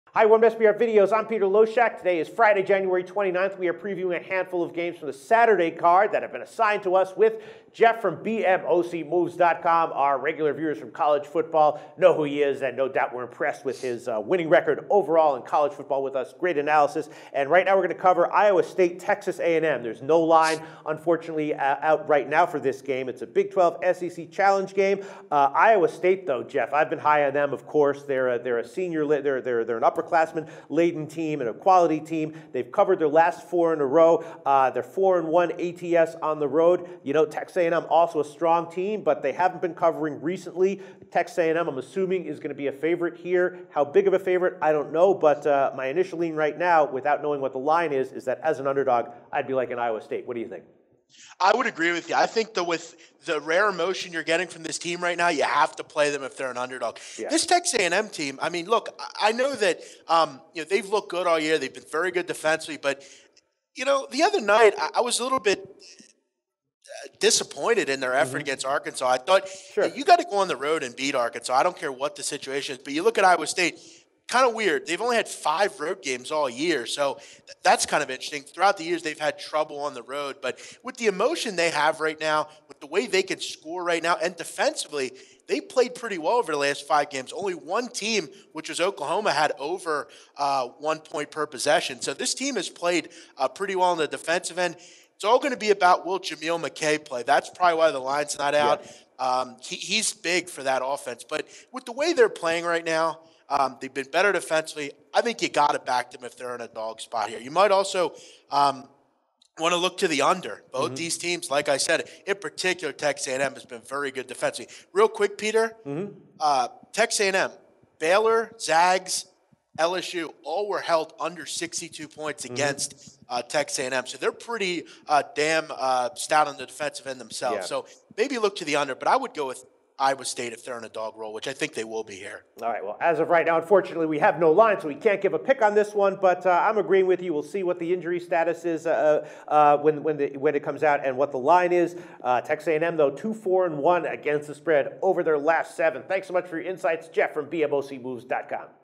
Iowa State vs. Texas A&M Big 12/SEC Challenge NCAA Basketball Picks
The Iowa State Cyclones and Texas A&M Aggies meet on Saturday January 30th in college basketball in a Big 12/SEC challenge game. Guest handicapper Jeff from BMOCMoves.com and SBR Videos host Peter Loshak discuss possible betting value on the game with the current market odds.